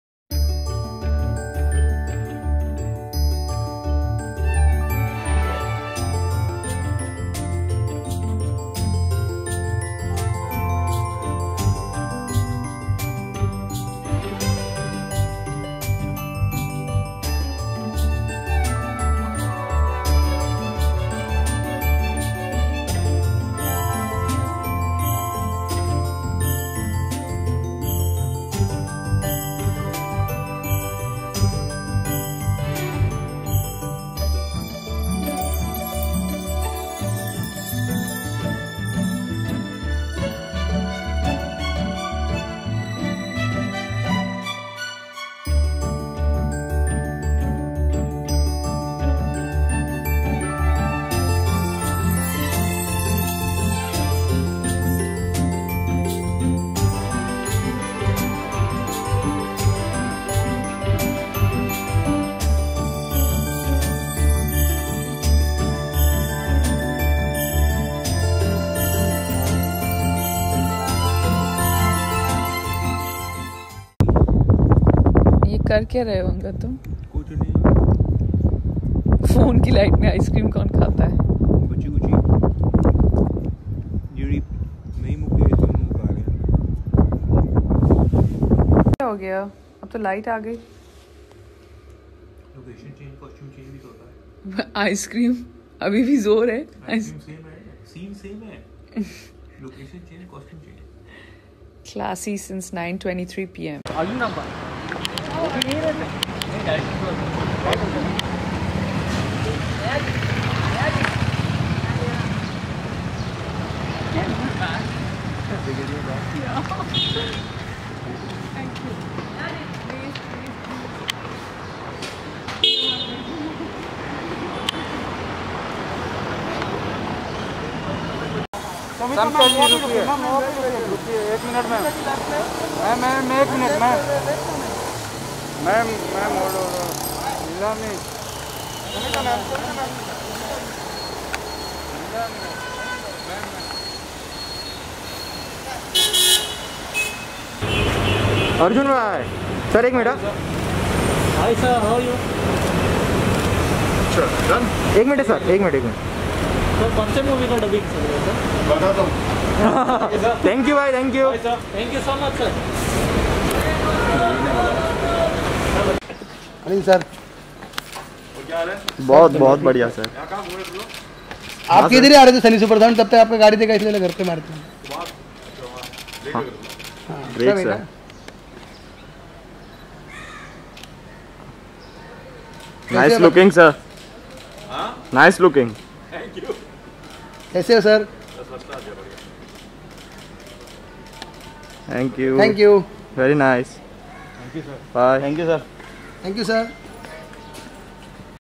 Neha Dhupia Daughter Mehr Drawing Cartoon With MOM & DAD In Maldives Island
Neha Dhupia In Maldives